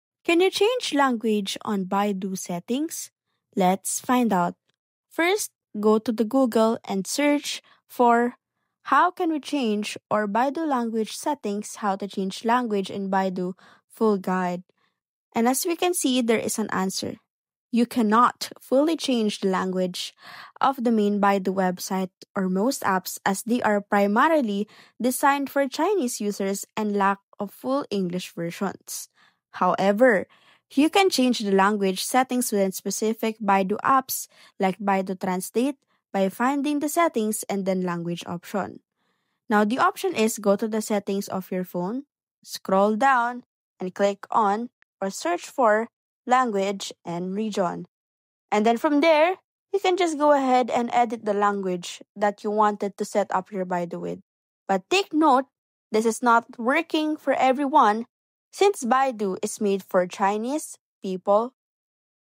Baidu Language Settings | How to Change Language in Baidu — Full Guide (Updated)
Learn how to change language settings in Baidu on iPhone and Android with this updated, easy-to-follow guide!